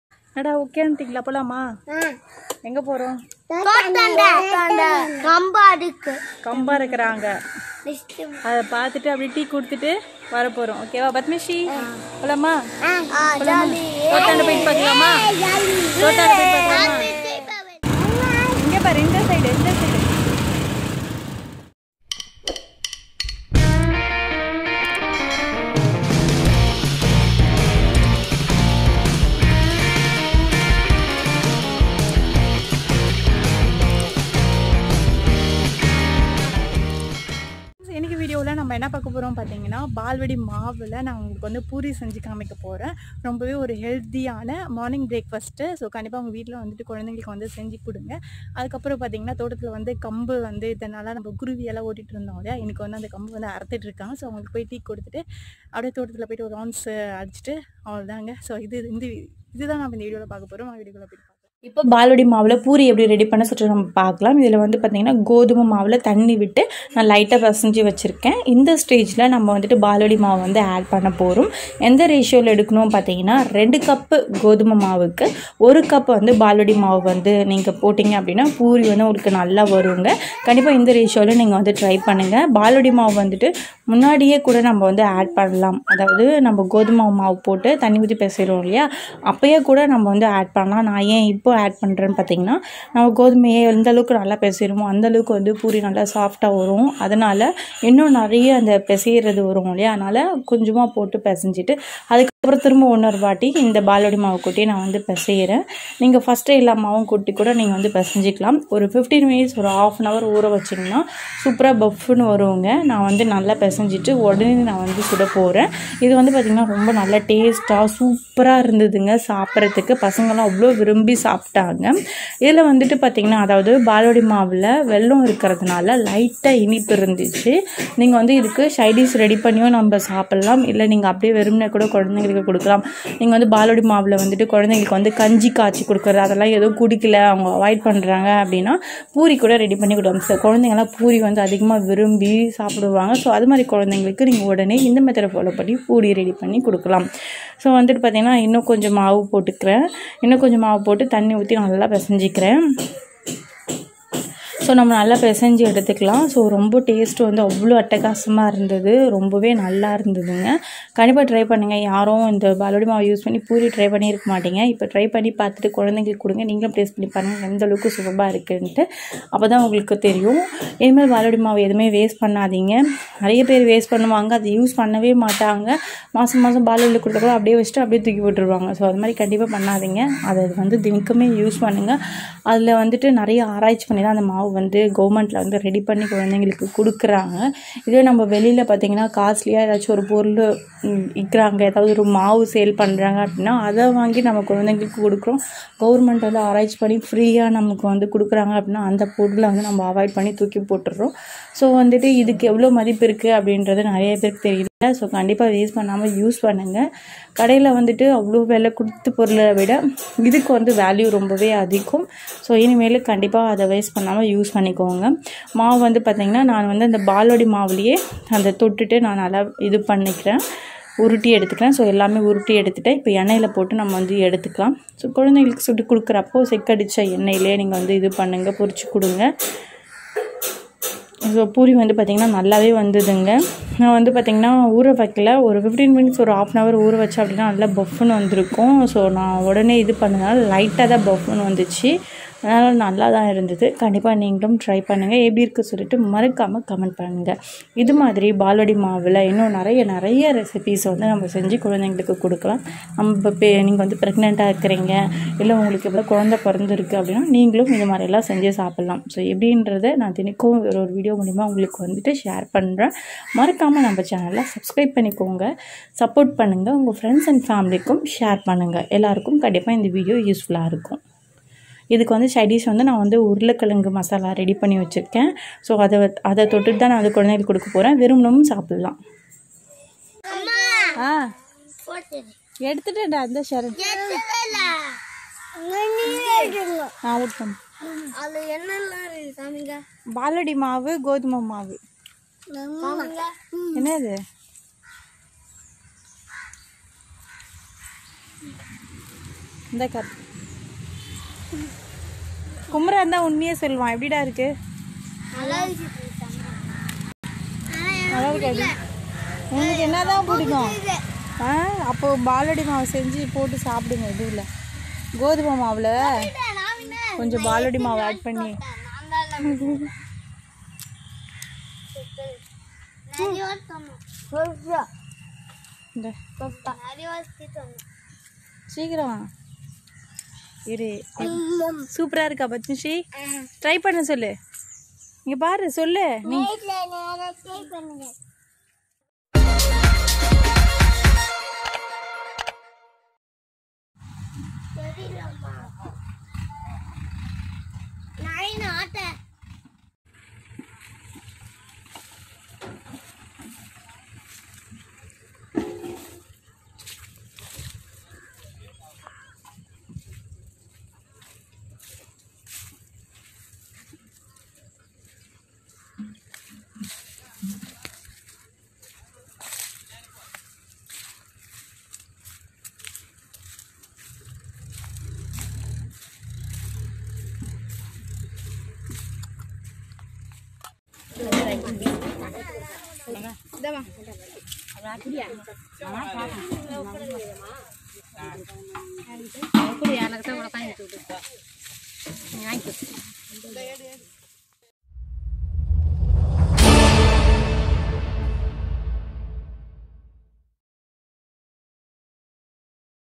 பால்வாடி மாவில் பூரி/தோட்டத்தில் நாங்கள்/கம்பு அருத்தல்/டீ .....
Instagram id
nam_ulagam
Sim Sim Usha store
please support and follow..